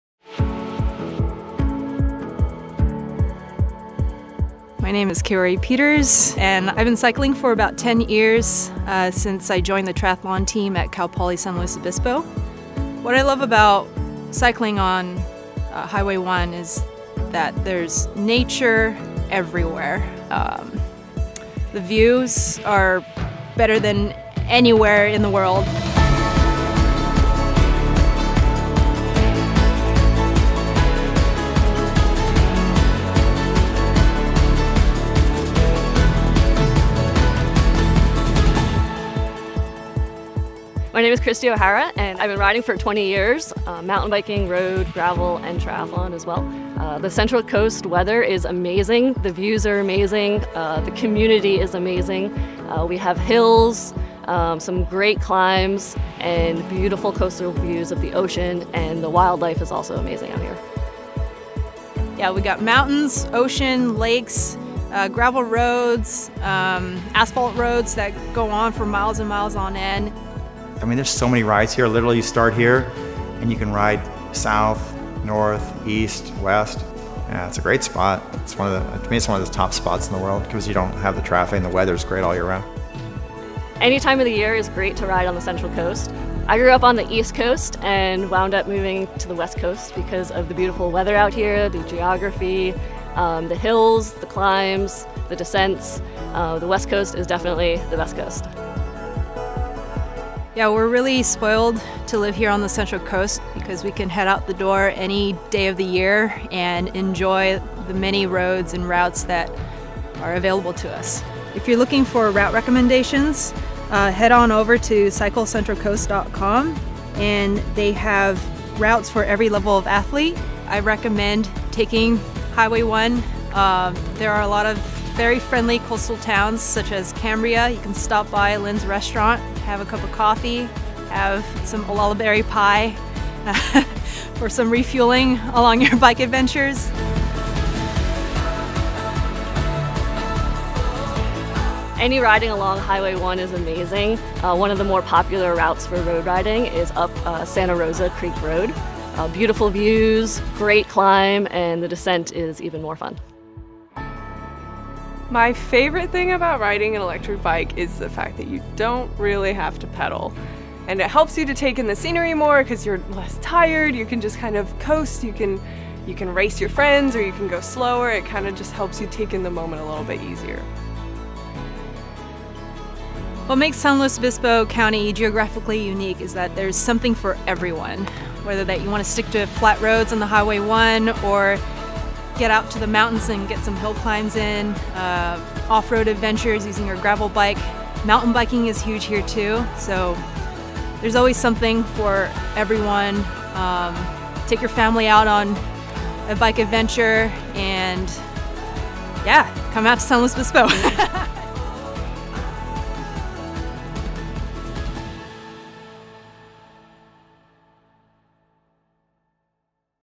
Highway 1 Cycling Routes along the Pacific Coastline
An all-time favorite cycling route, the San Luis Obispo County section of Highway 1 is a stretch representing the very best of the West Coast. The 57 miles between Ragged Point and Nipomo pass over coastal bluffs, beside mountain ranges, through beach towns, nature preserves, and vineyards. No wonder it’s become a favorite section on the Pacific Coast Route.

Through San Luis Obispo County, Highway 1 generally follows the Pacific coastline, with just a couple of brief (and totally worthwhile) exceptions. These also happen to lie along perfect backroads for making a long-distance ride. Advanced cyclists may want to clip in for one or more of the intense routes on Cycle Central Coast or want to compete in one of our many cycling events.

But you don’t have to be a pro or a die-hard cyclist to enjoy pedaling on our stretch of Highway 1. Bring your bike and ride it to the neighboring town, or to the nearest mountain bike trail — or just to the ice cream shop. Each of the destinations along our slice of Highway 1 has its own cycling routes and bike trails, from family-friendly to advanced.